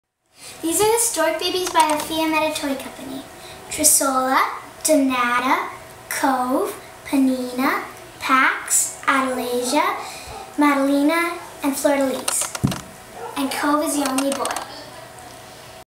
Stork Babies
Starla P introducing Stork Babies by The Fiammetta Toy Co. Inc.